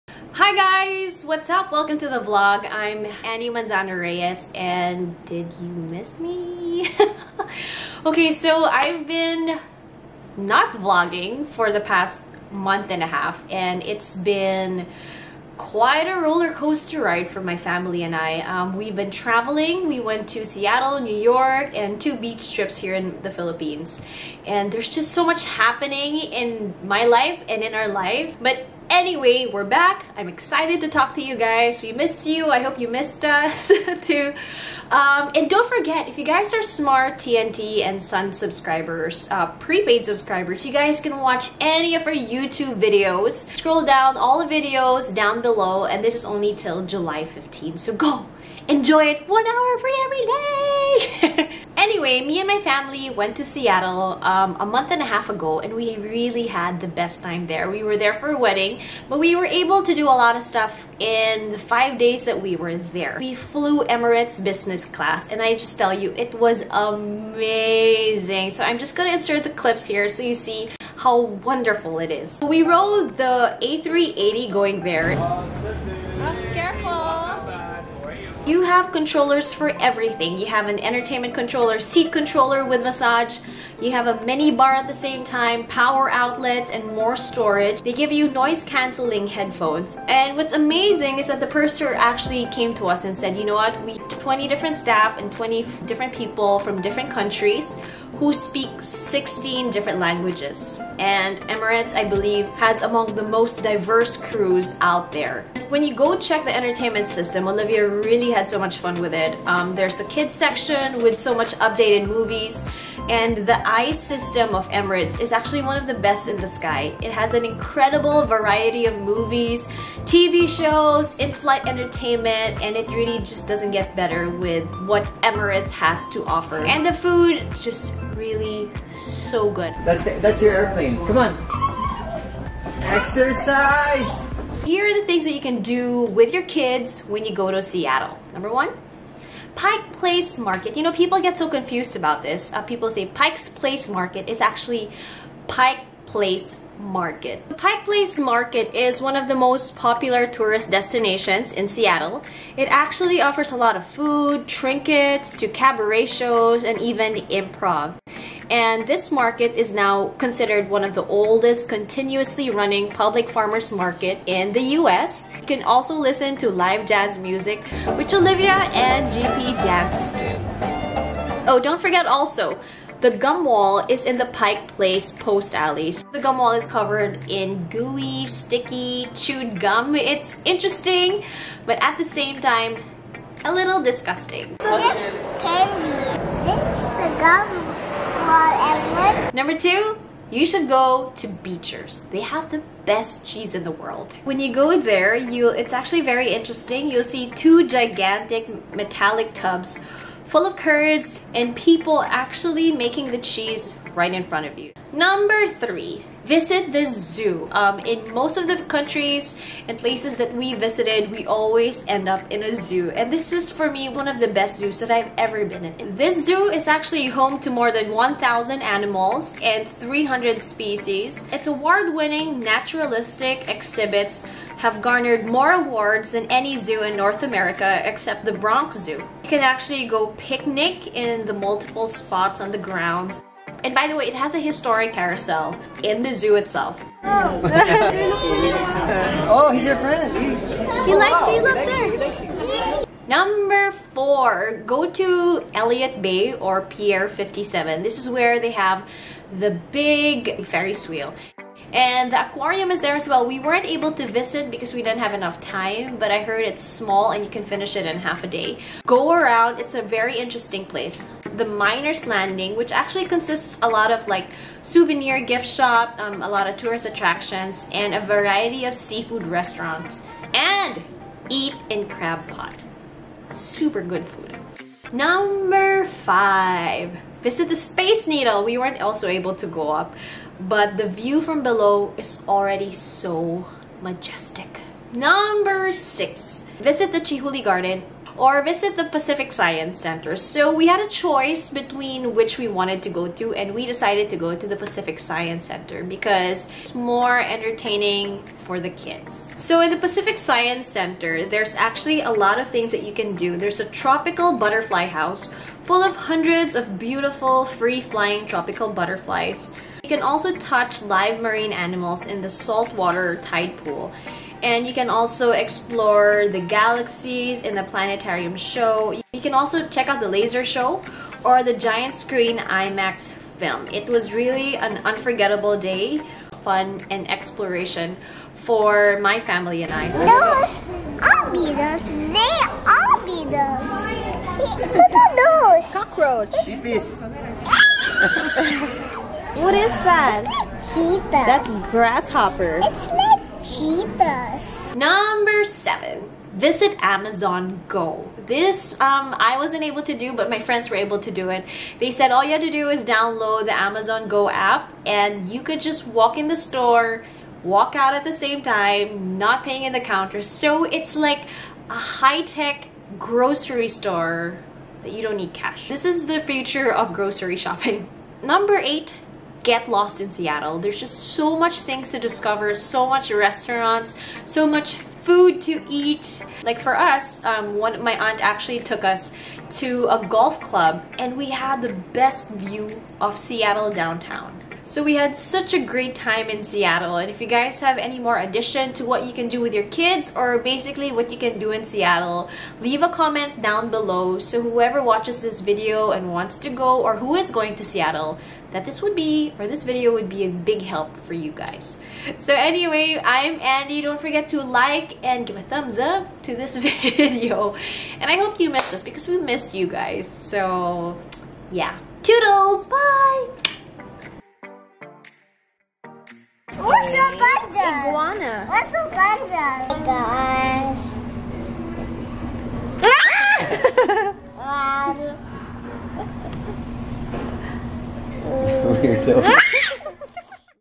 SEATTLE VLOG -- Pike Place Market, Gum Wall, Space Needle & Highlights || Andi Manzano Reyes
Here are the Top Things to do In Seattle for us! We had such a great time and can’t wait to go back and explore even more especially the places we missed.